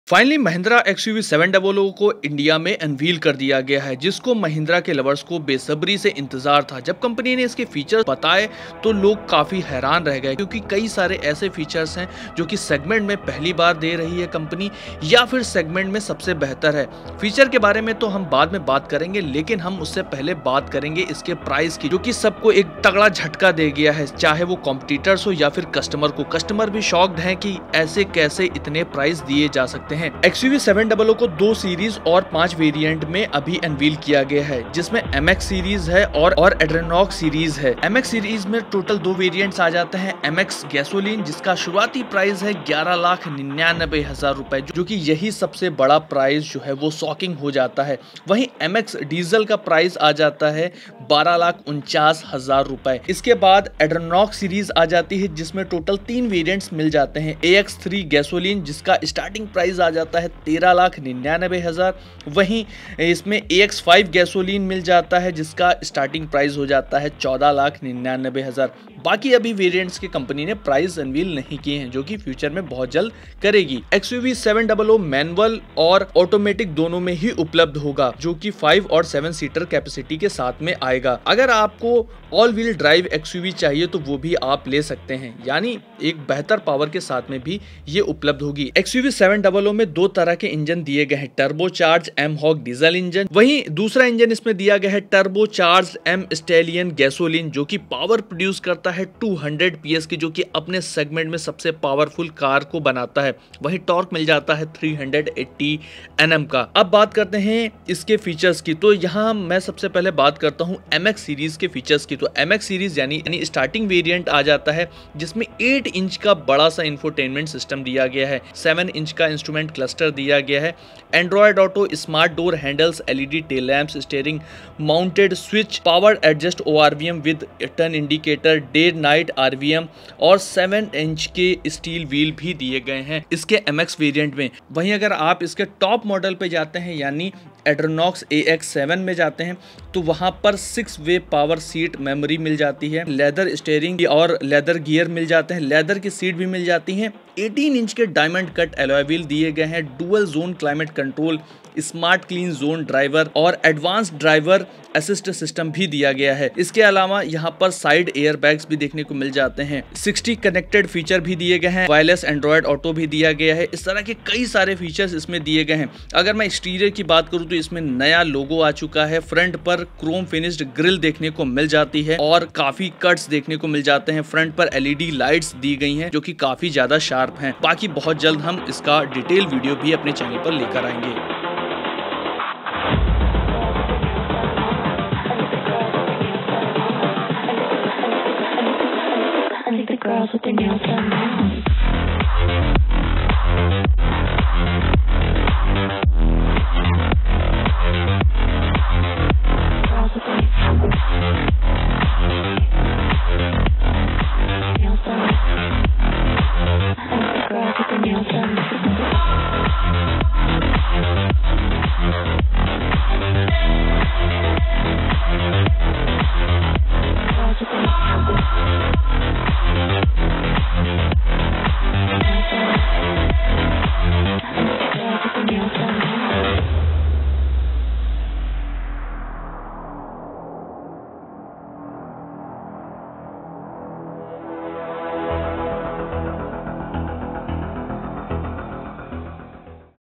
Mahindra XUV 700 Price | Features
The Mahindra XUV700 is the latest SUV that has been unveiled in India and it comes after a lot of anticipation. the Mahindra XUV700, even before it was officially unveiled, had developed a fan following of itself. Mahindra has been able to do get this much attention on the XUV700 as it has been spotted testing in a camouflaged avatar and during the past few days, seen undisguised too. Additionally, Mahindra has been constantly teasing out details about the upcoming Mahindra XUV700, including things like the features and comfort that the XUV700 will come with, creating a massive interest in the upcoming SUV.